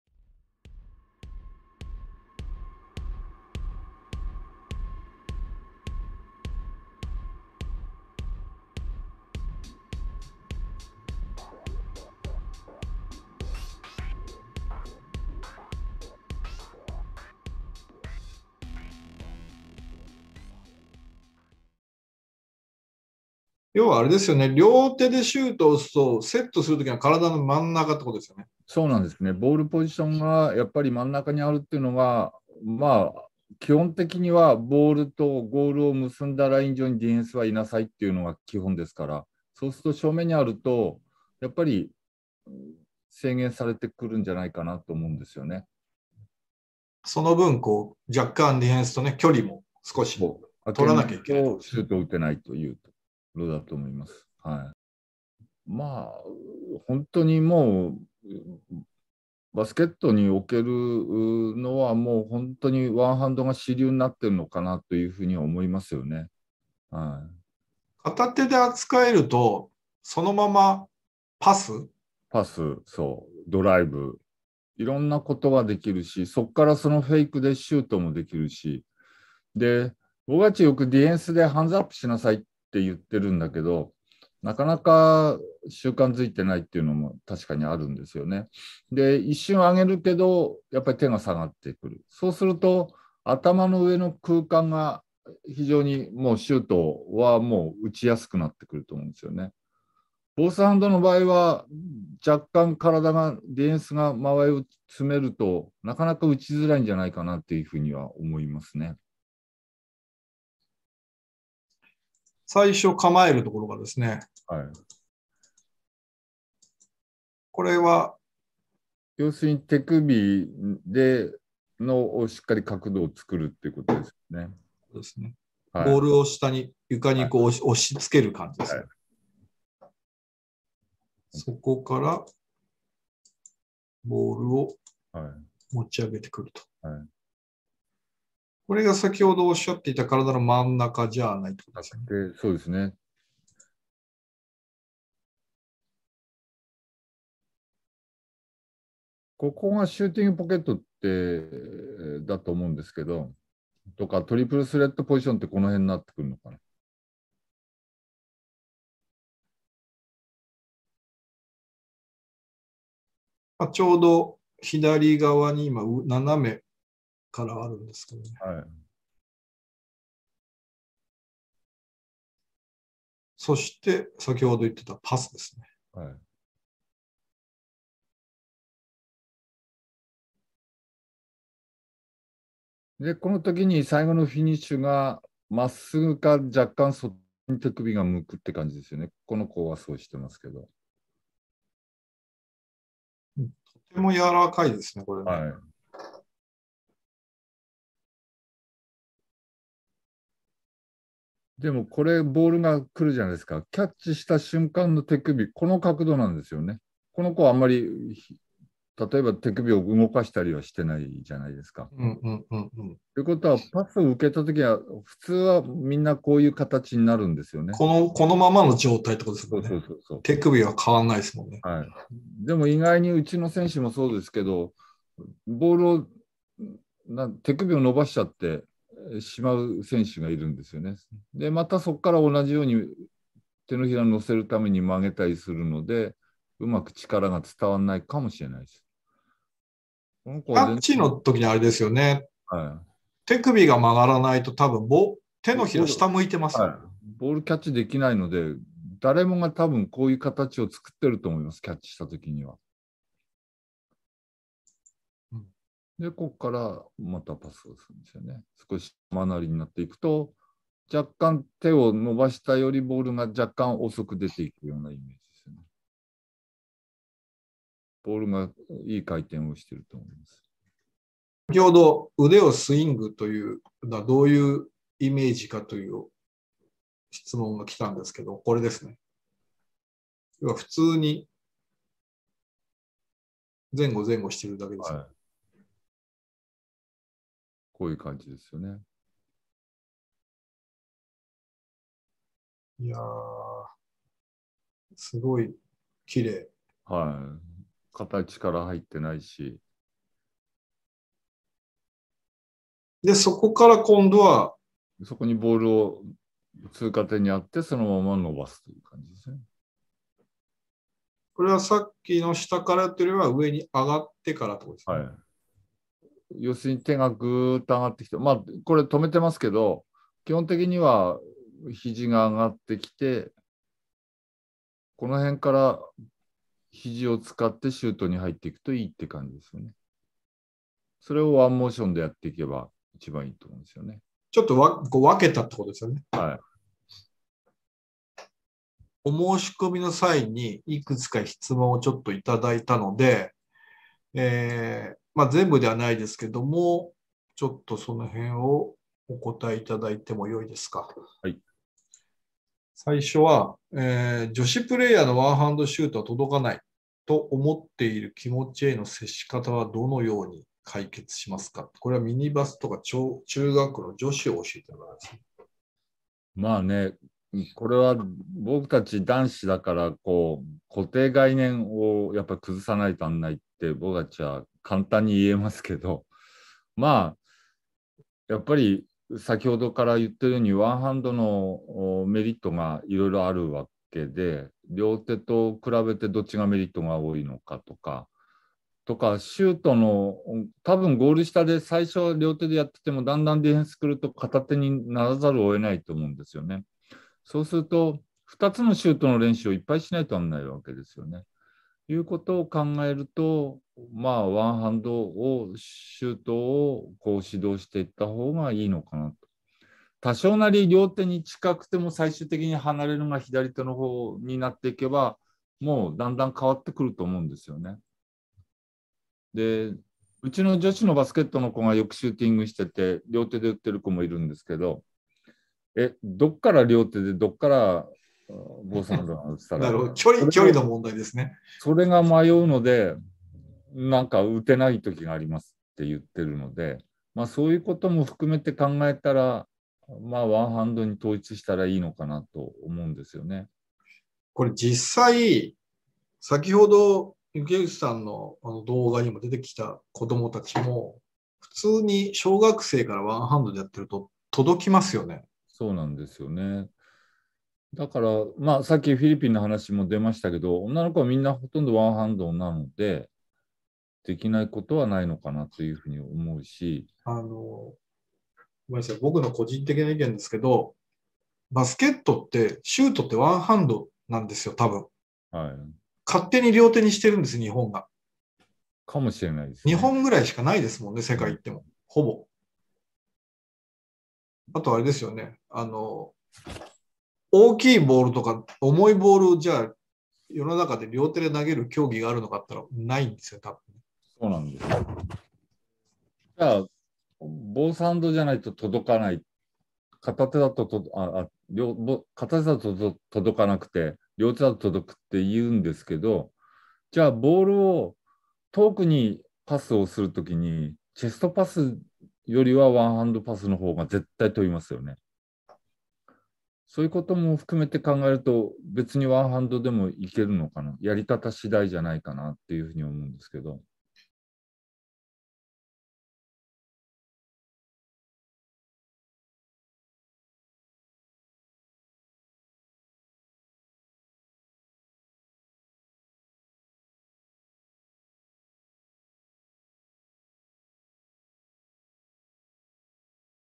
小・中学生でも無理なくできるワンハンドシュート
ワンハンドシュートのフォームを作る方法をレクチャーします